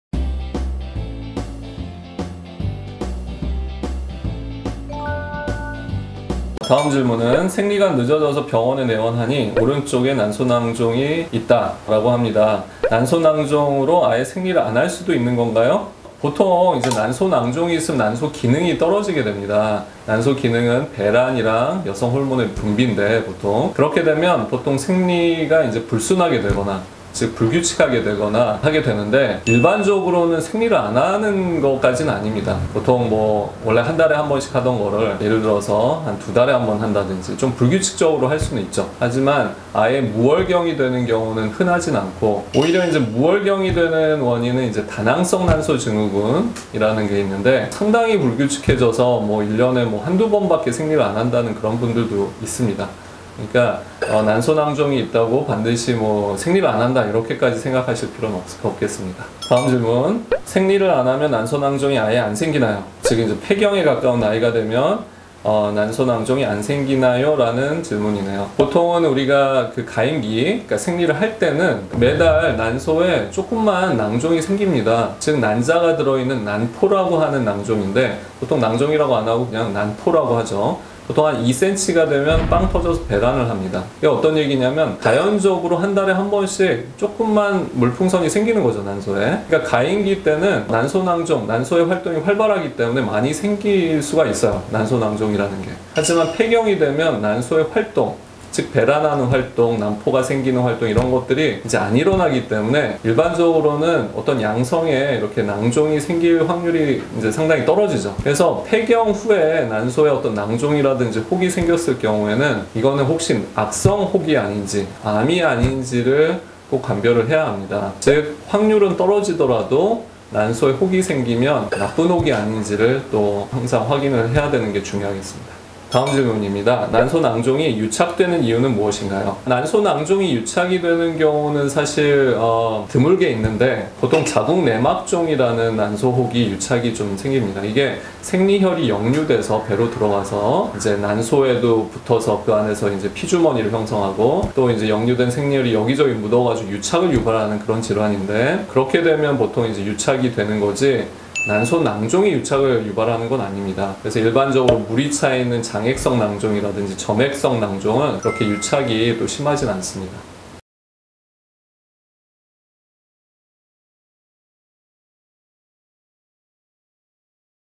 [최궁금] Q. 폐경기 난소낭종, 암일 수도 있다구요?
💌소중한 구독자 님의 질문이 도착했습니다💌

언택트로 안전하게! 전문의가 정확하게!
자궁난소 대표 지킴이 최동석 원장님과 함께하는 시간,
"최원장님, 궁금해요!"

최궁금으로 들어온 오늘의 질문!
Q. 생리가 늦어져서 병원에 갔더니 난소낭종 있대요
난소낭종으로 생리를 아예 안할수도 있나요?
Q. 폐경기에 발견한 난소낭종, 암일 수도 있다구요?
Q. 난소낭종이 유착되는 이유는 무엇인가요?

내.몸.내.알. 내 몸은 내가 알자!
최 상산부인과와 오늘도 내 몸 알기 함께해요'-^

- 위치 : 서초대로 351 (교대역 4번출구 강남역 방향)